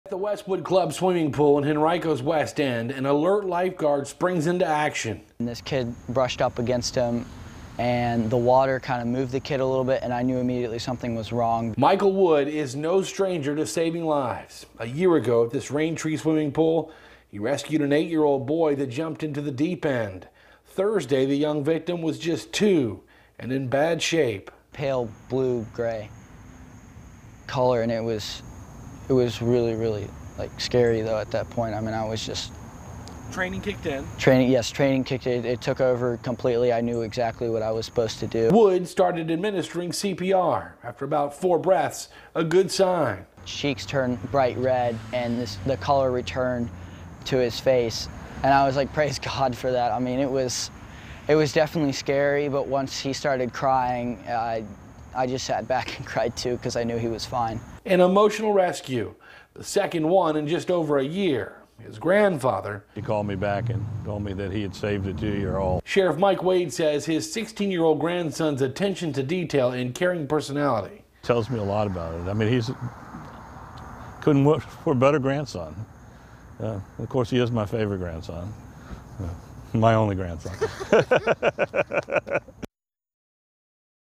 'He Started Crying, I Started Crying Too': Teen Saves Drowning Toddler at Virginia Pool
Michael Wood, 16, rescued a toddler and brought him back to life Thursday at the Westwood Club swimming pool in Henrico after the child nearly drowned.